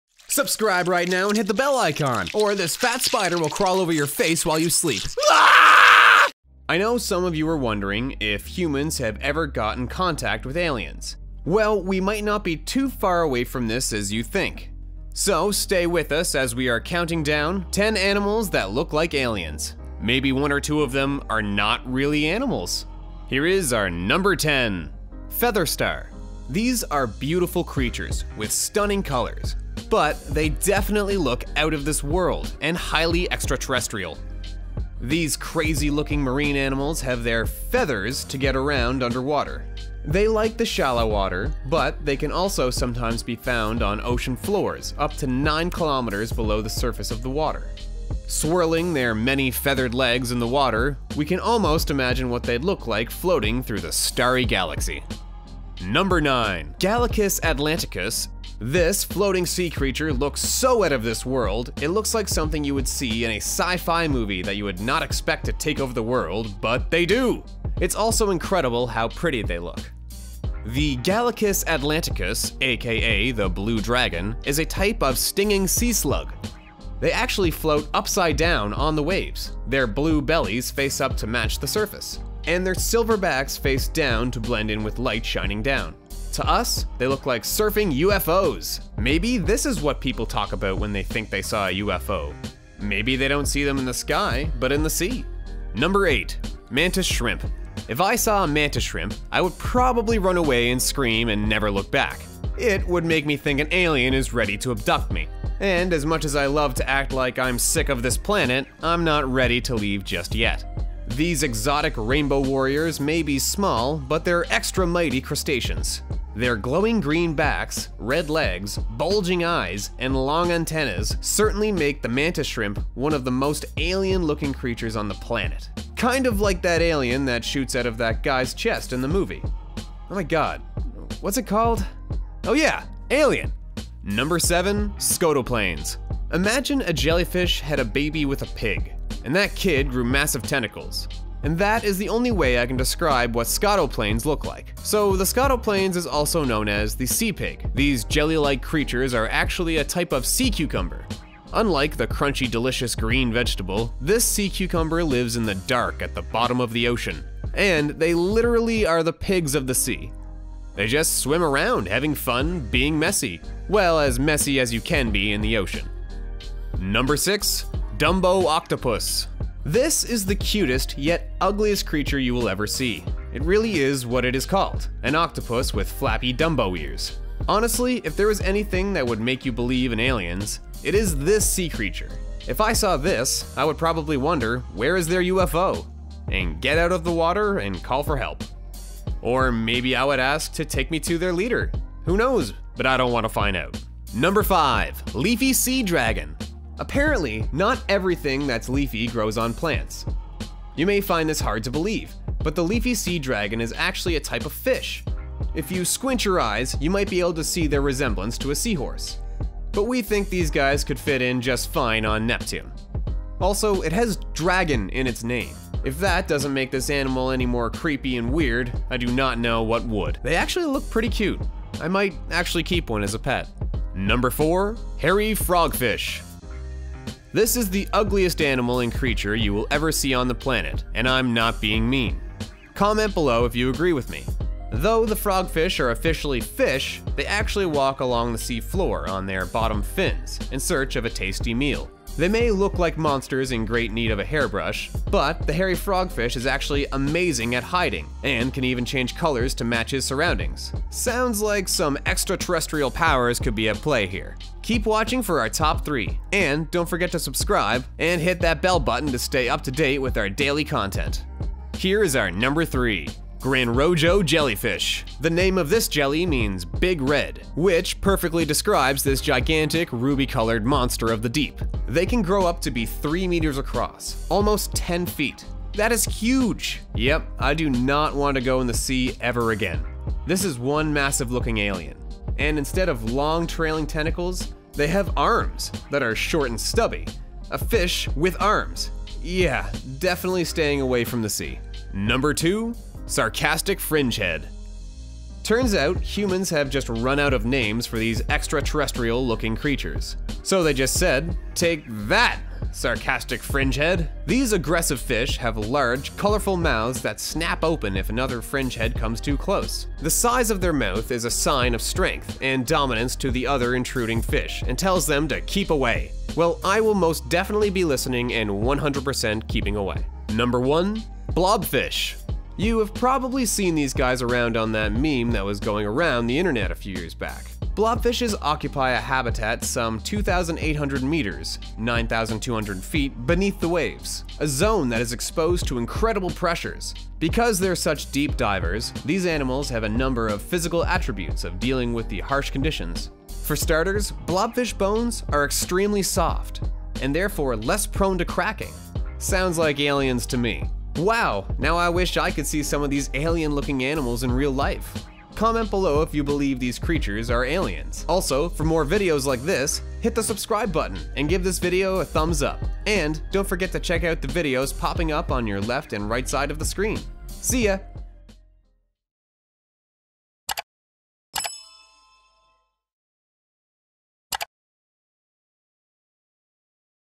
10 Animals That Look Like Aliens
I know some of you are wondering if humans have ever got in contact with aliens? Well we might not be too far from this as you think. So stay with us as we will be counting down „10 animals that look like aliens“. Maybe one or two of them are not really animals.